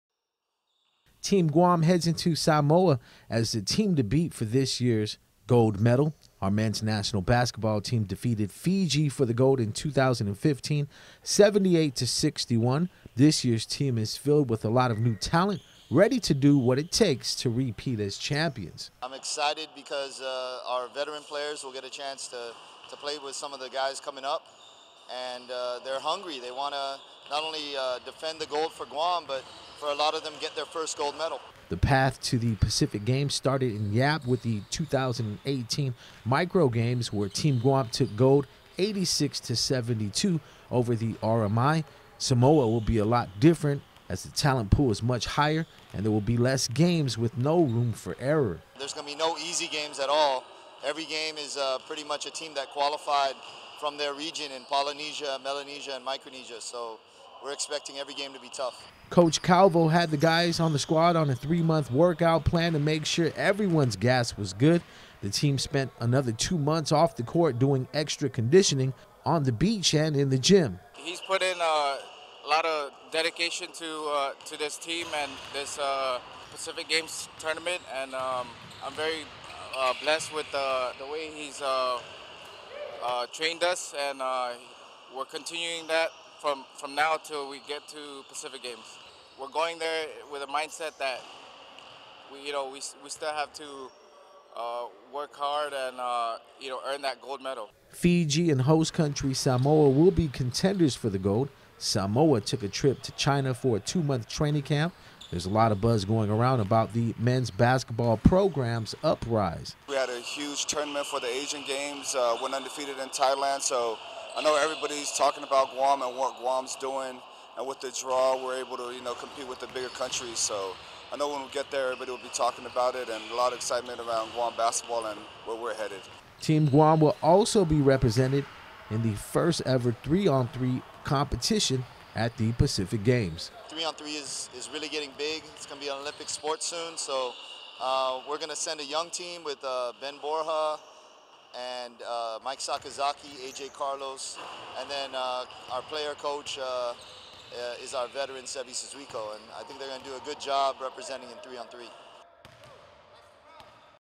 Pacific Games preview: men's basketball
Team Guam heads into Samoa as the team to beat for this year's Pacific Games gold medal. Our men's national basketball team defeated Fiji for the gold in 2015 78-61. This year's team is filled with a lot of new talent, ready to do what it takes to repeat as champions.